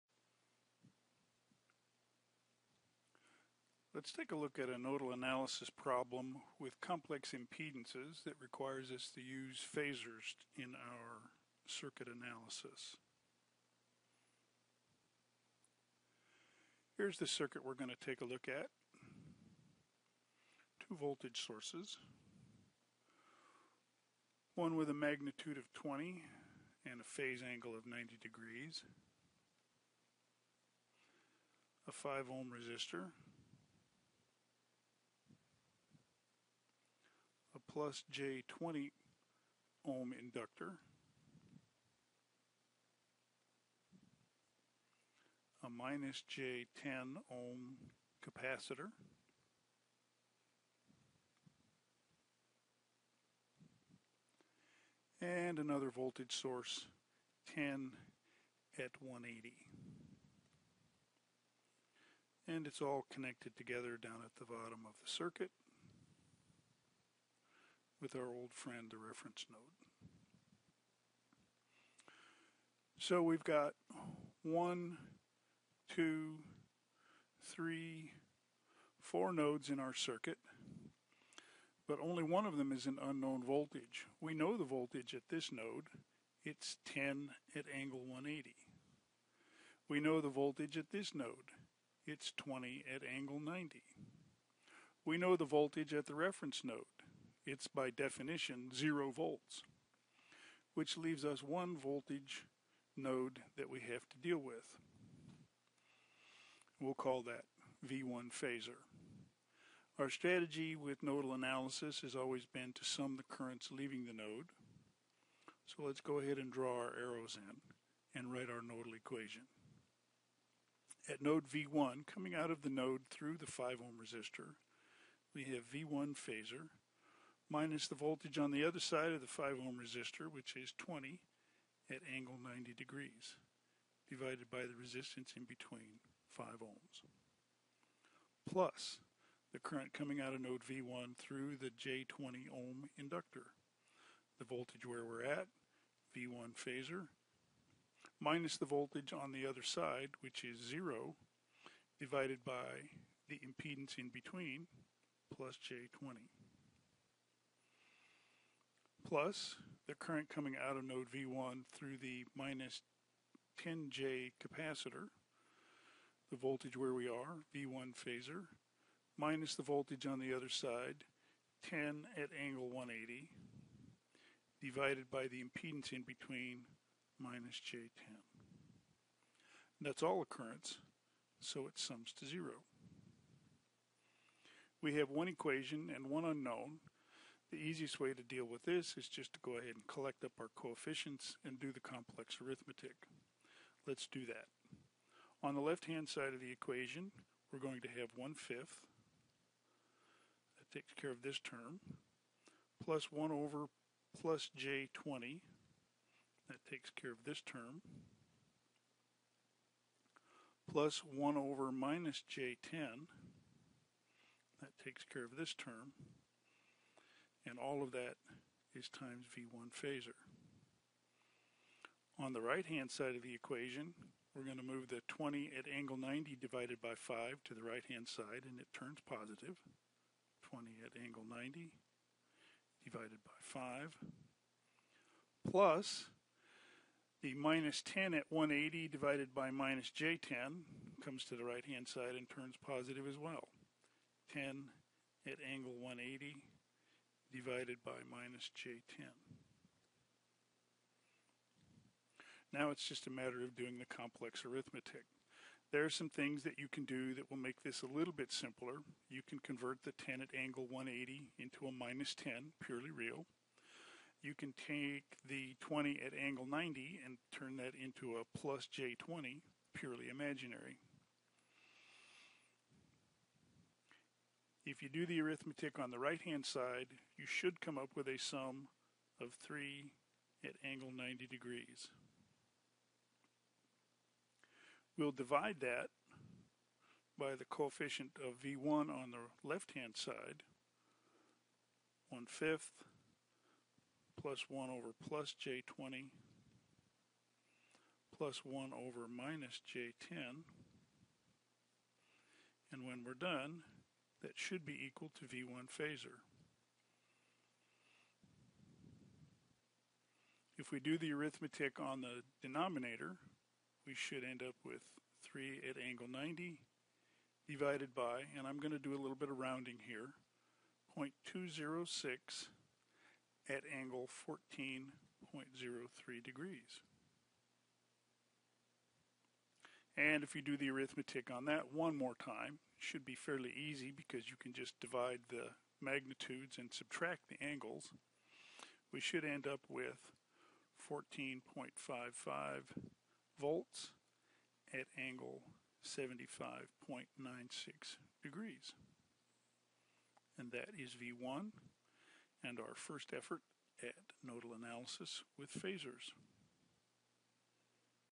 nodal_analysis_with_phasors.mp4
A nodal analysis problem with complex impedances and voltage sources